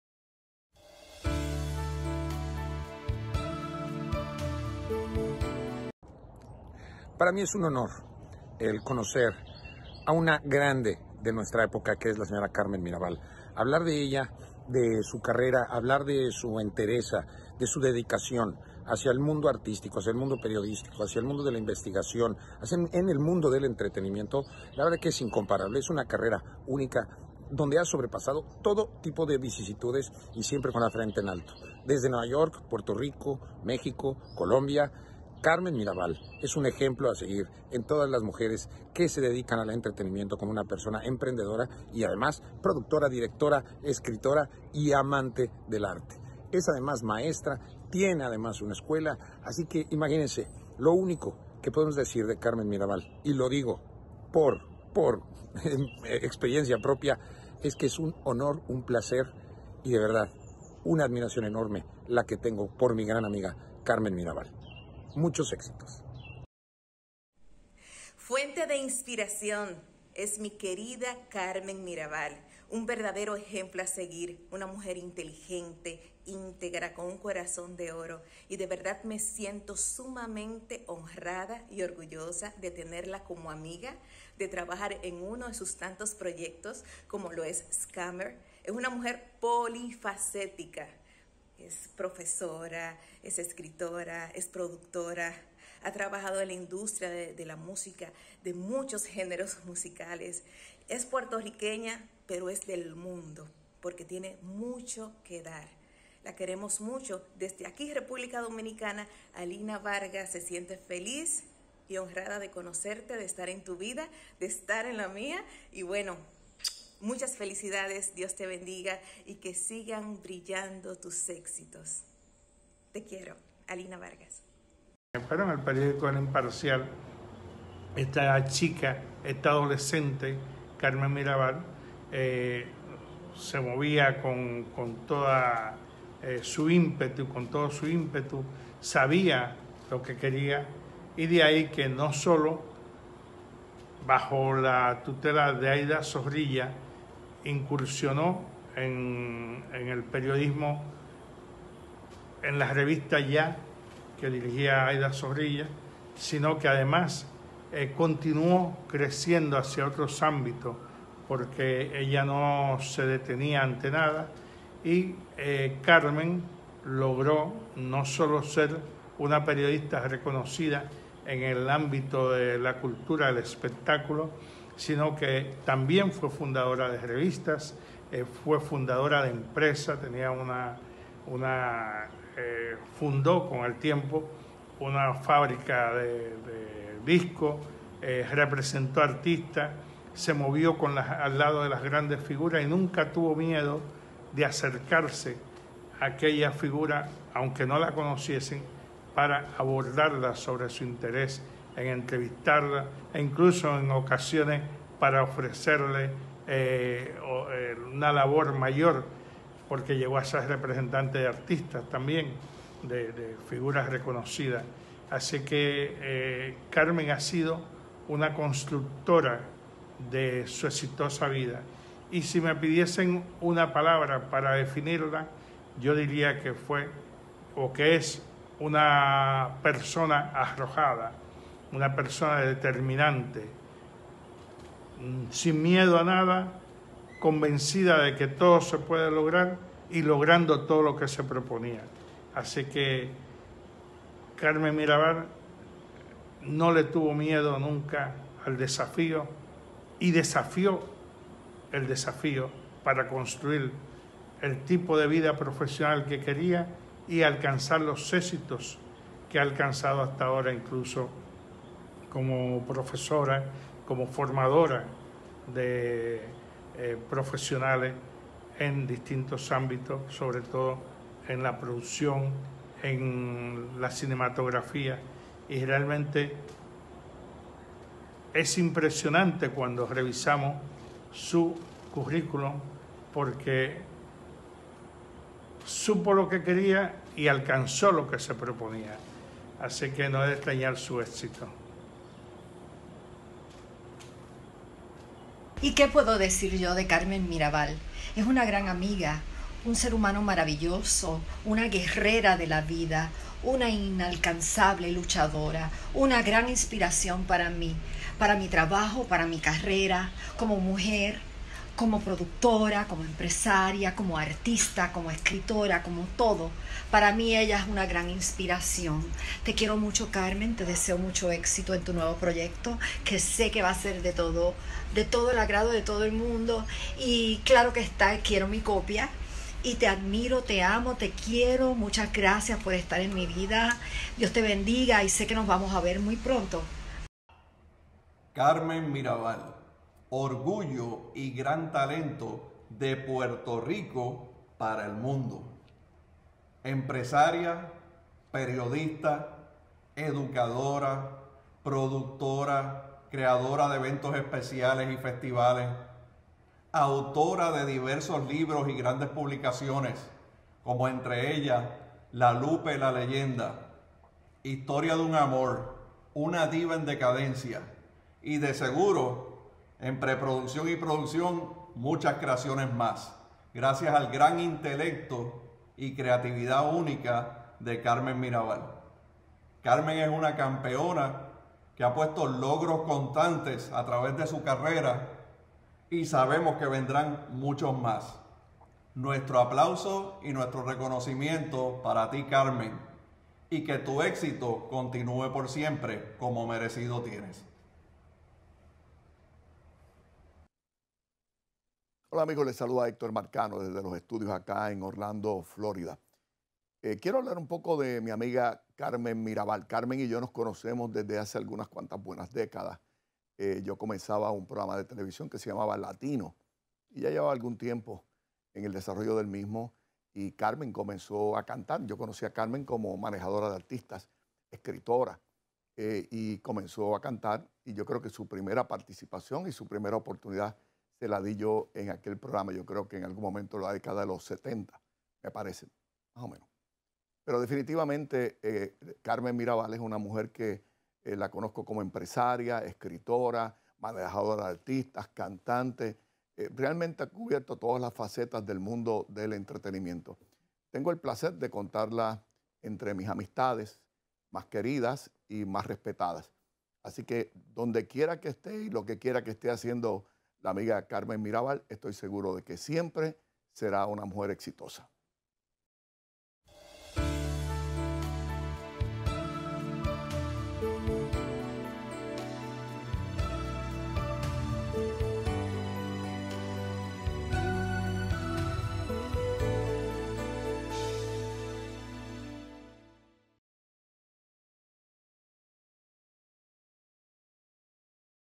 Profesionales hablan de La Mirabal
Mark Tacher (actor y productor), Alina Vargas (cantante y actriz), Nelson del Castillo (reconocido periodista), Denissa (cantante y productora), Heri Martinez de Dios (Vice presidente de Atlantic University) y Héctor Marcano (Animador y productor de TV).